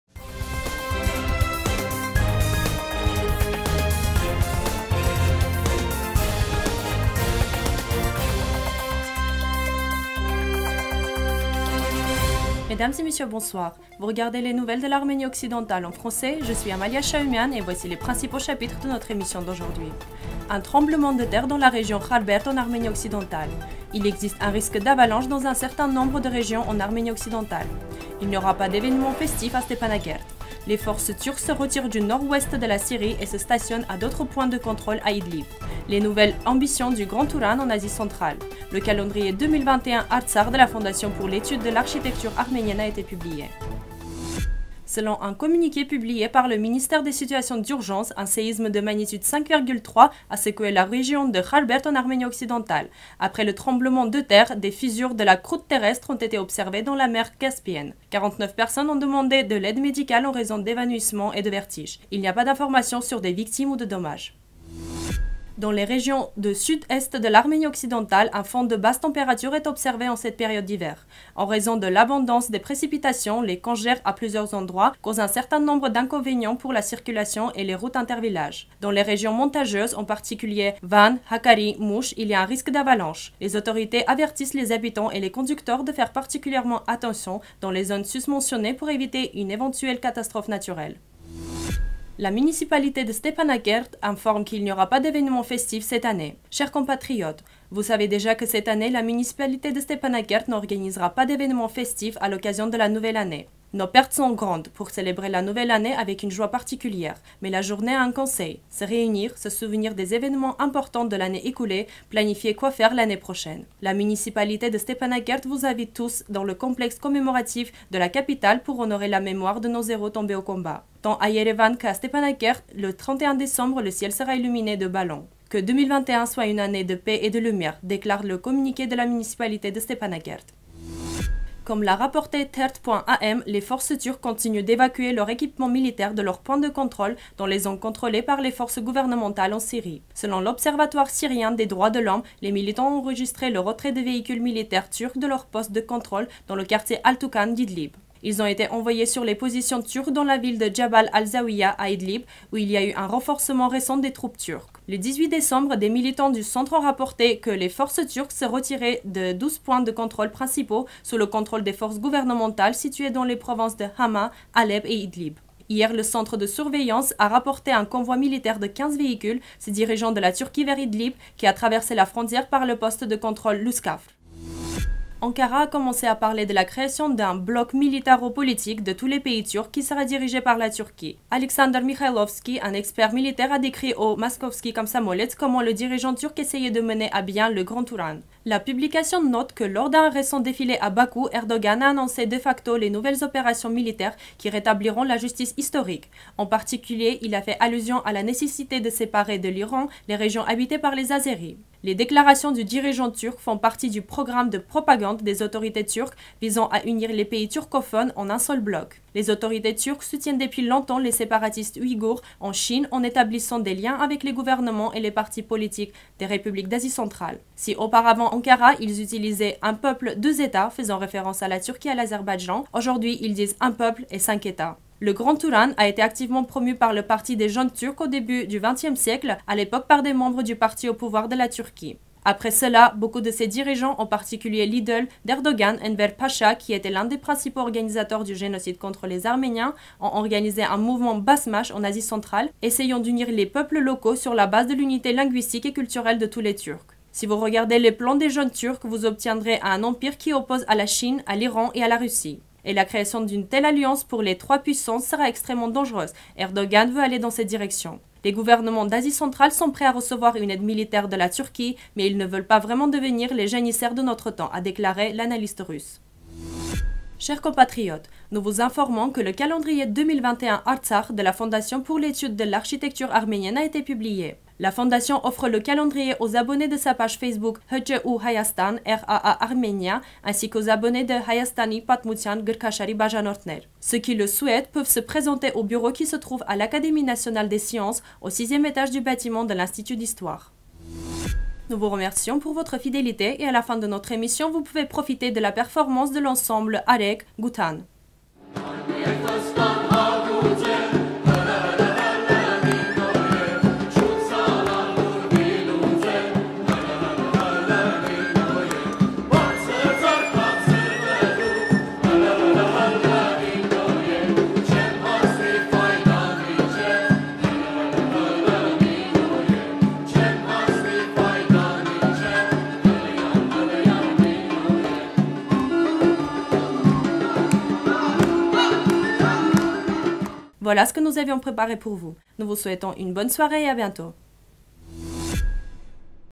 Kharberd en Arménie Occidentale.Nouvelles 28.12.2020
1. Un tremblement de terre dans la région Kharberd en Arménie Occidentale
2. Il existe un risque d'avalanches dans un certain nombre de régions en Arménie Occidentale
3. Il n'y aura pas d'événements festifs à Stepanakert
4. Les forces turques se retirent du nord-ouest de la Syrie et se stationnent à d'autres points de contrôle à Idlib
5. Les nouvelles ambitions du "Grand Turan" en Asie centrale
6. Le calendrier 2021 "Artsakh" de la Fondation pour l'étude de l'architecture arménienne a été publié